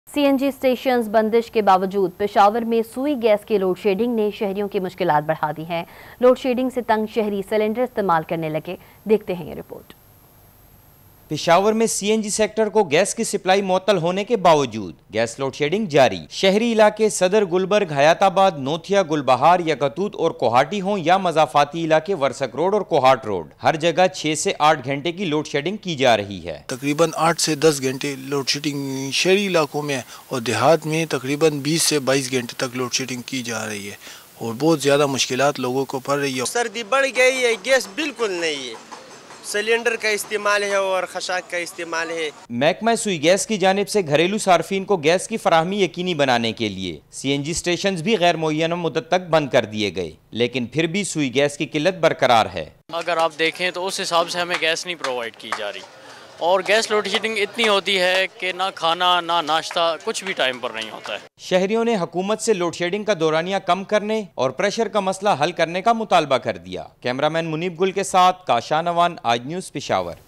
Peshawar: Despite the closure of CNG stations, gas load shedding continues - Aaj News
Gas supply to CNG stations in Peshawar is suspended  - 07 January, 2024 - Aaj News

Spot Light With Munizae Jahangir:
shorturl.at/gwFU6